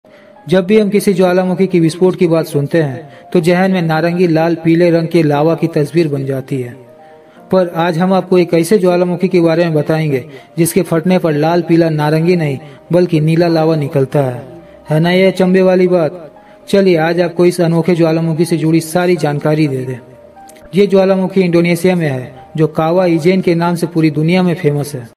kawah ijen || blue volcano || nila jwalamukhi || for full video check in description
ye volcano indonesia me hai .
photographer - Olivier Grunewald

biggest acidic lake in the world